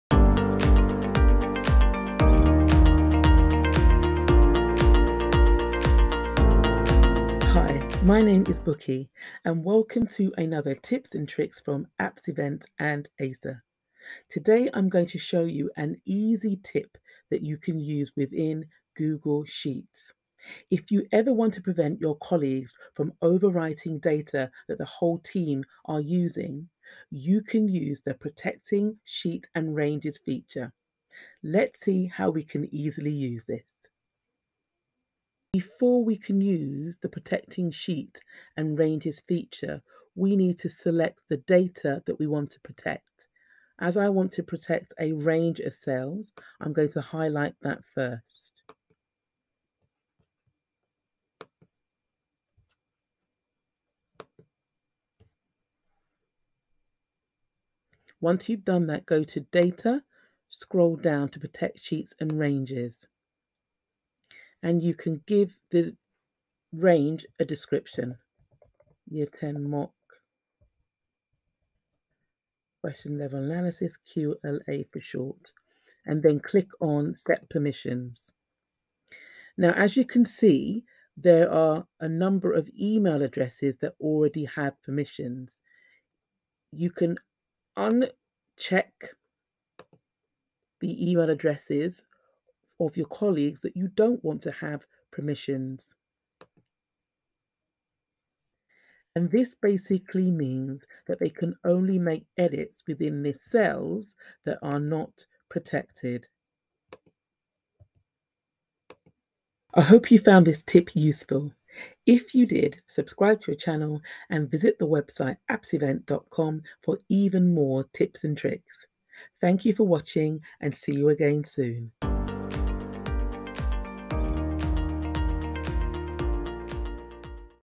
Google Sheets - Protecting sheets and ranges | Tips & Tricks Episode 123
Using the 'protecting sheets and ranges' feature in Google Sheets, will mean that no data can be accidentally overwritten.

➤➤➤➤Get your own Online training with AppsEvents here: AppsEvents.com/online